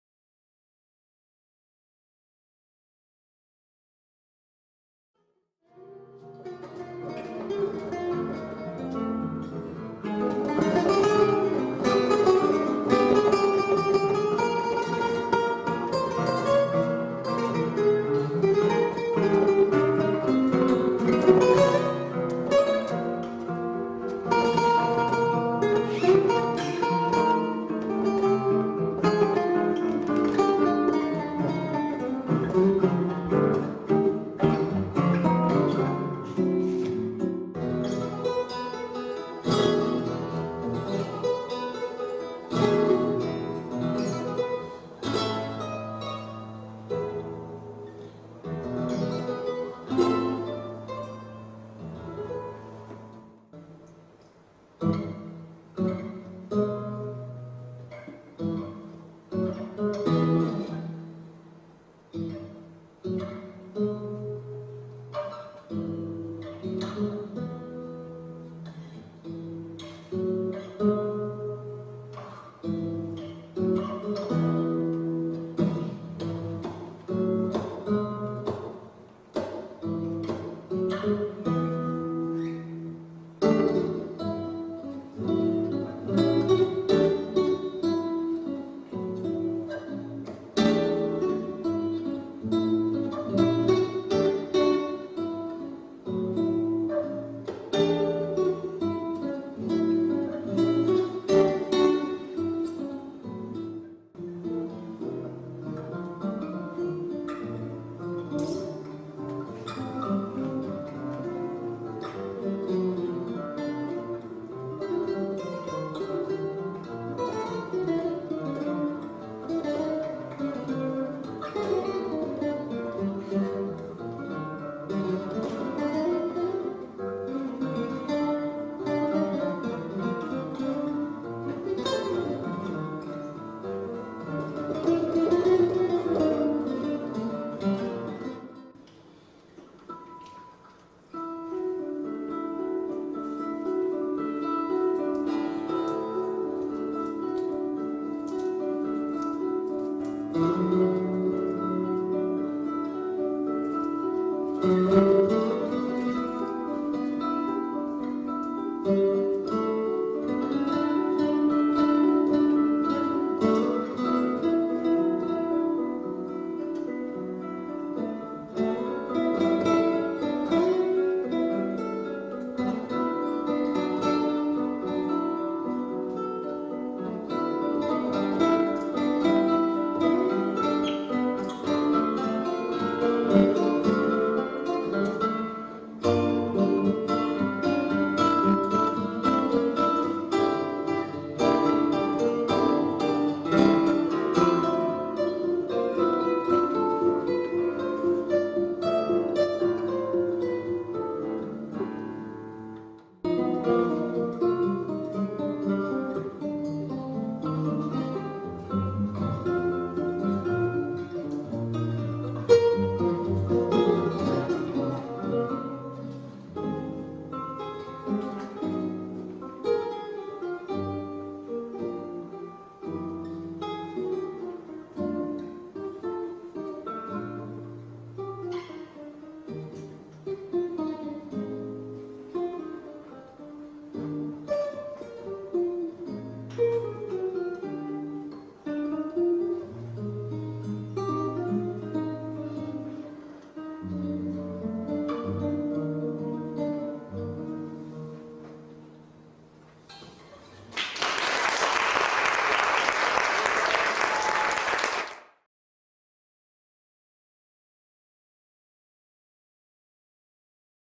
2019点と線シリーズ2 東京公演 PV
名古屋を拠点に全国で演奏活動を展開しているJazz Guitarist
Tony Nori氏とClassic Guitarist Miura Hiroshi によるのSolo&Duo Concert
シリーズ「点と線」第2回東京公演のためのPV